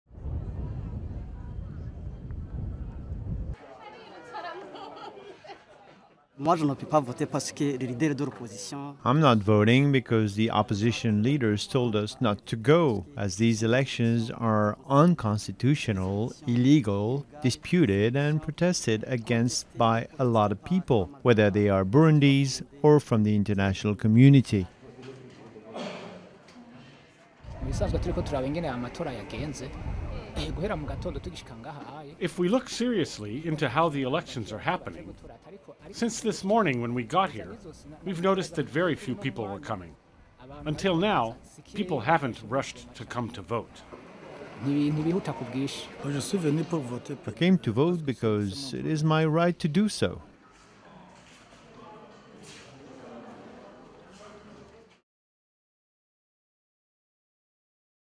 Burundi elections marred by low participation and violence
Burundi's presidential election has been marred by violence, international criticism and low participation, with President Pierre Nkurunziza heading towards a victory filled with threat for the country's future, still bearing the marks of a long civil war.